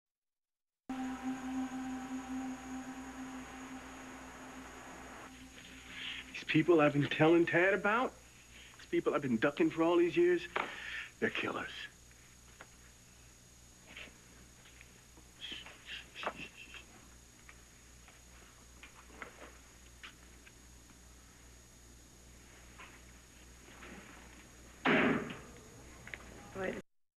Tad hears someone at the door...
of the cabin. it's Frankie...